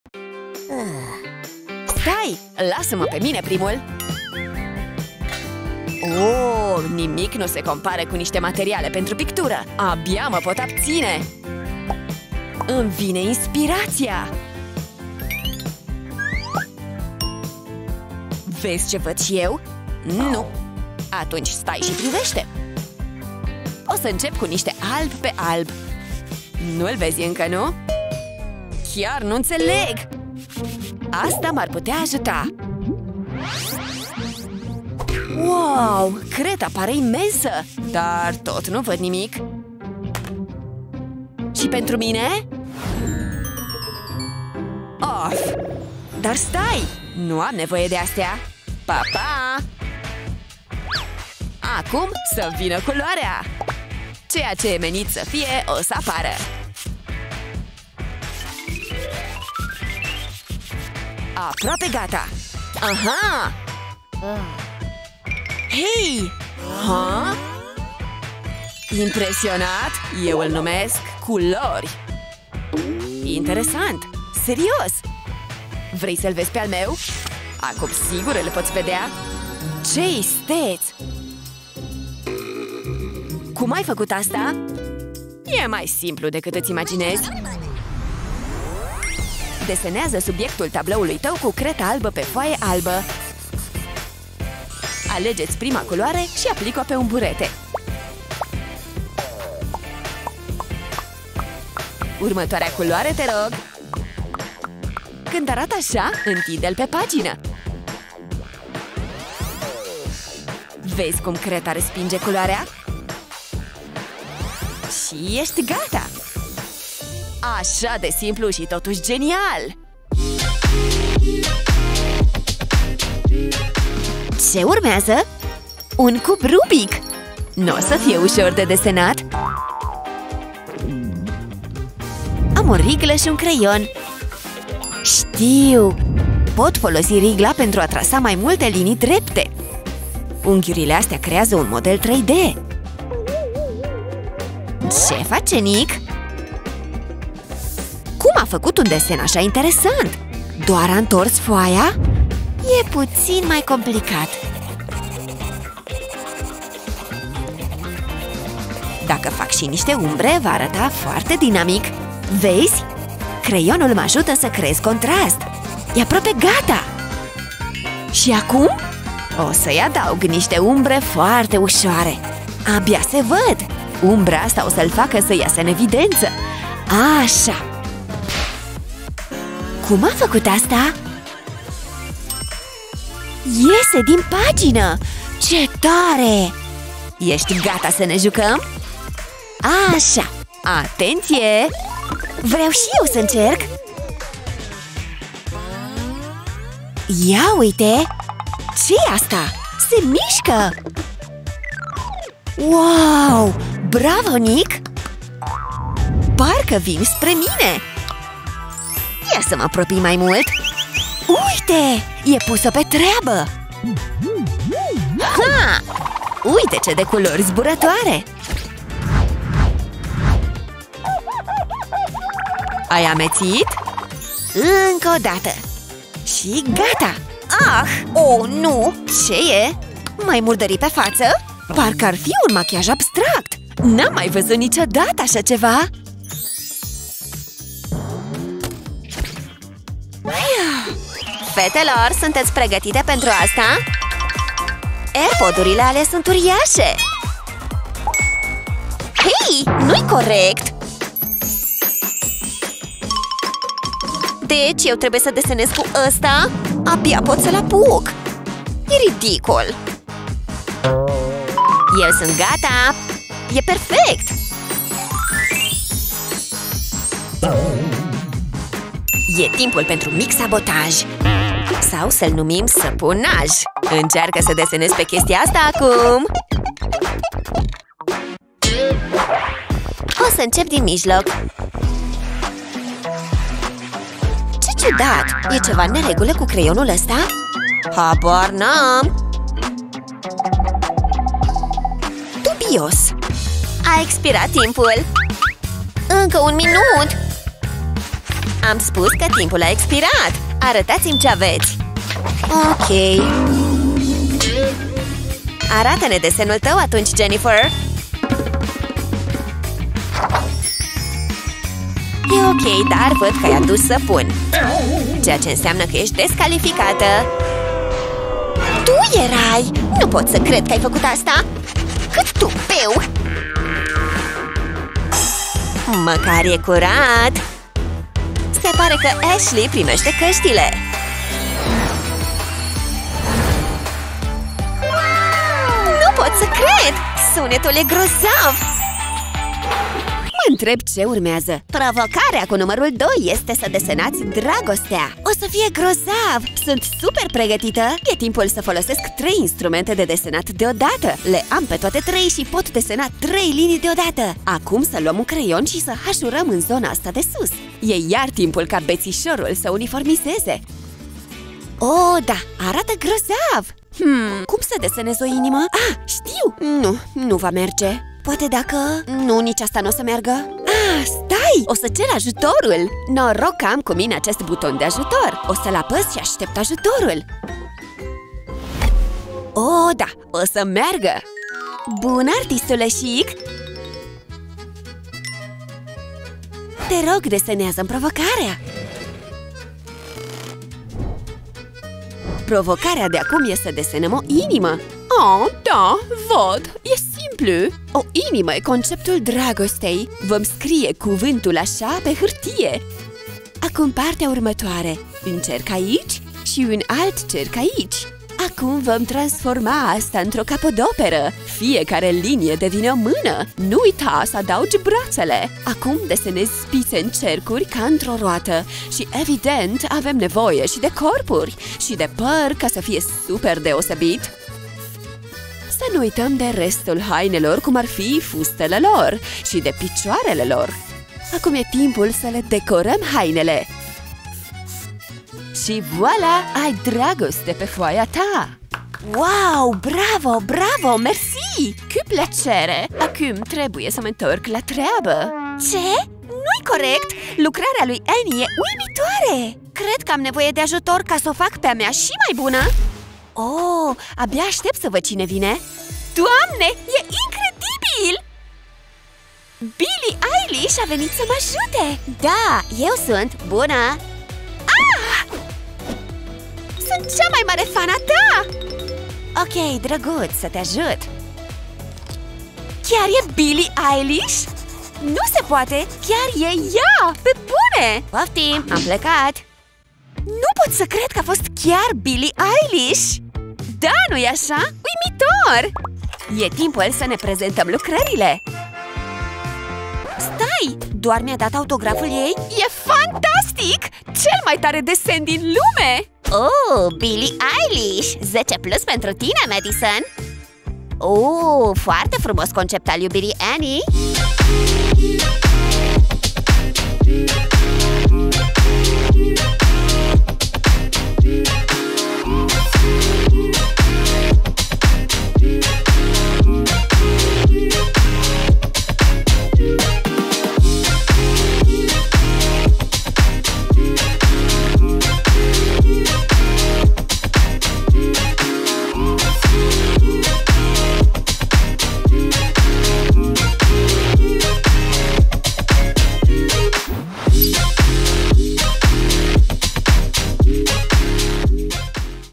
PROVOCARE ARTISTICĂ ȘI TRUCURI PENTRU DESEN || Cele mai bune sfaturi creative, marca 123 GO! GOLD
Și ție îți place să desenezi? Hai să desenăm ceva împreună! Urmărește noul nostru clip video, în care ți-am pregătit o serie de noi provocări artistice. Distracție plăcută!